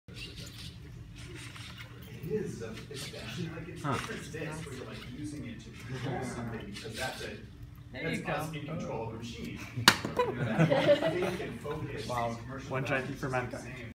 Neuro-Powered Robotics (Ultracortex + OpenBCI)
A participant at the NYC Media Lab's annual summit uses EMG artifacts recorded by the OpenBCI Ultracortex to control a robotic hand!